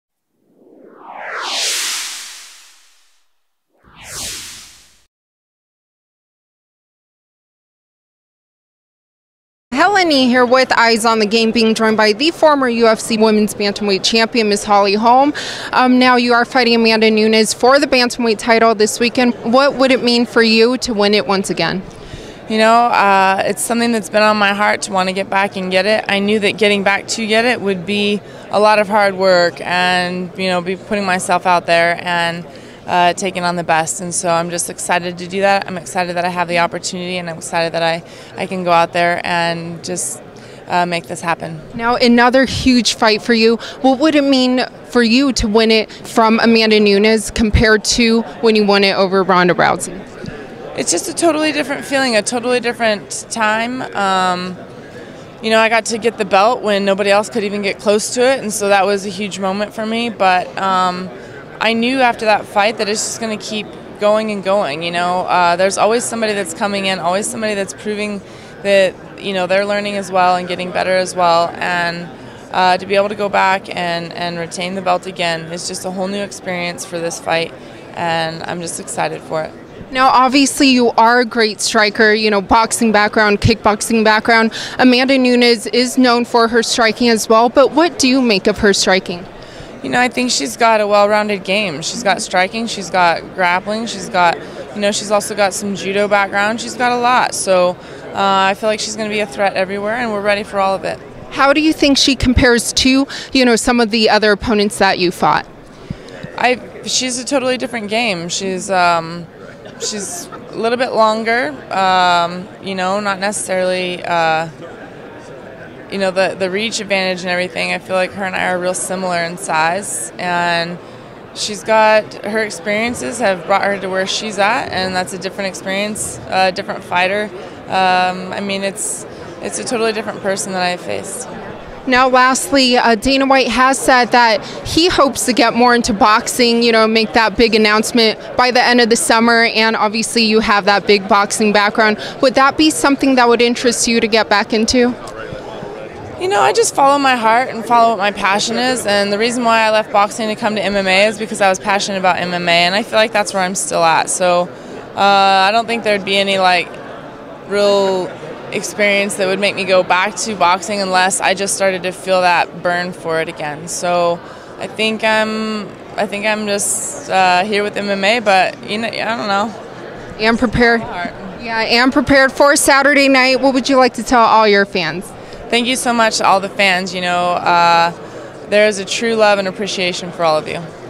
HOLLY HOLM ON AMANDA NUNES; IF SHE'D BOX FOR DANA WHITE'S PROMOTION
During UFC 239 media day, former UFC women's bantamweight champion Holly Holm spoke to Helen Yee of Eyes On The Game.
LVSportsNetwork.com